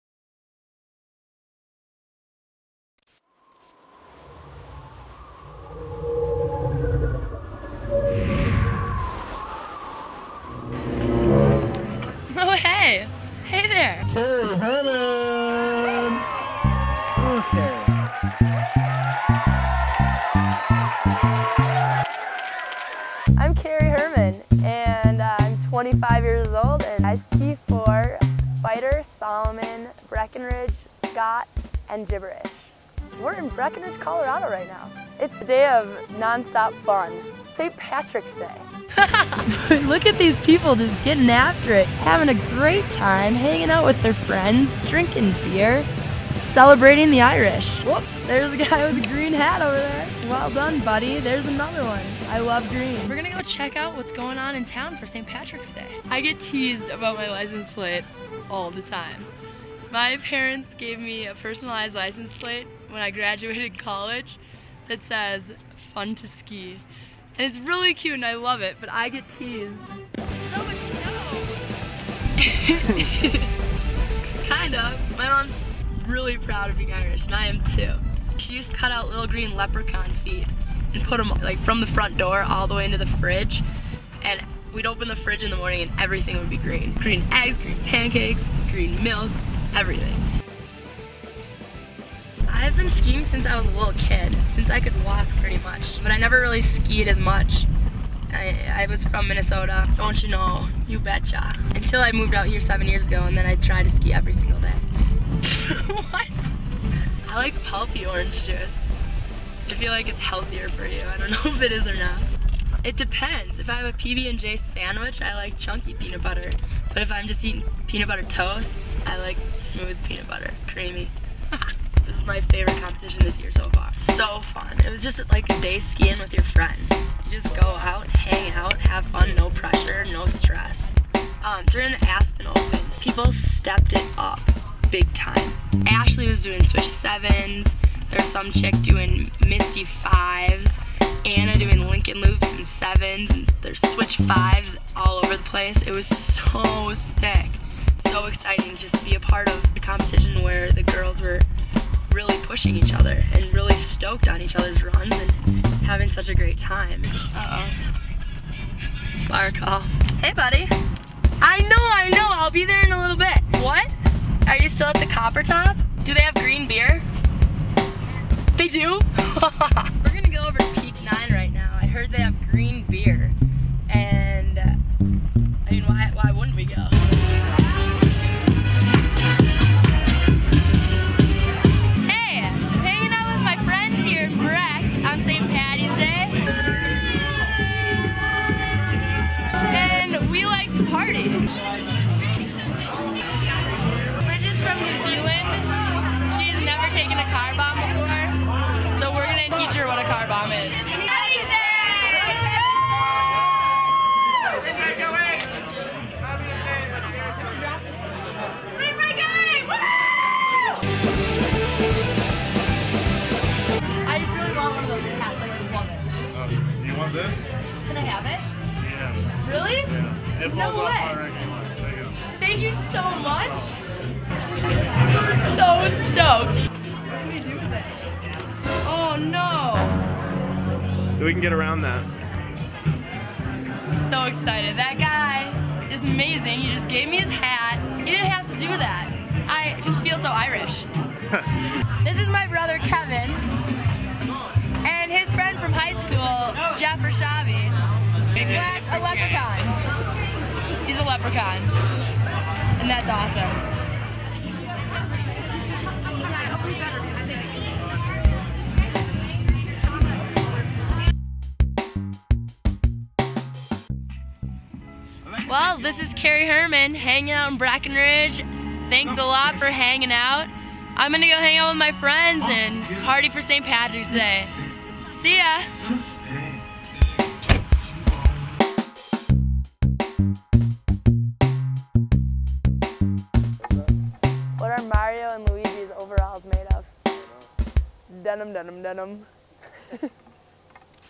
Keri Herman Spyder Profile
Keri sheds a little light on skiing and lets us follow her around on St. Patty's Day.